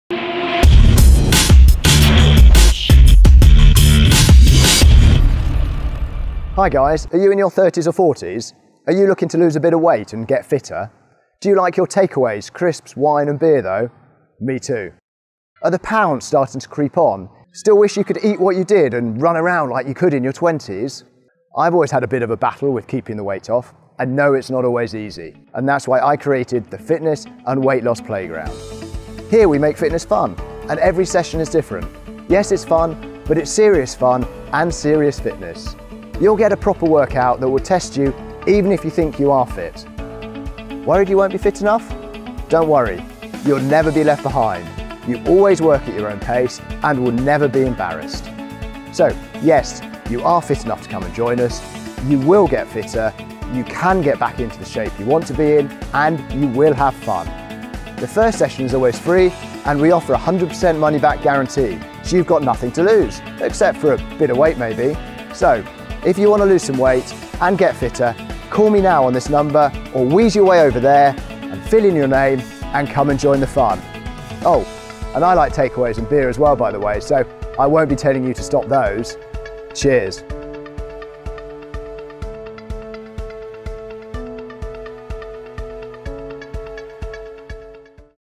Fitness & Weightloss Playground
Chichester based fitness company puts the fun into fitness to help lose weight and increase physical fitness.  For details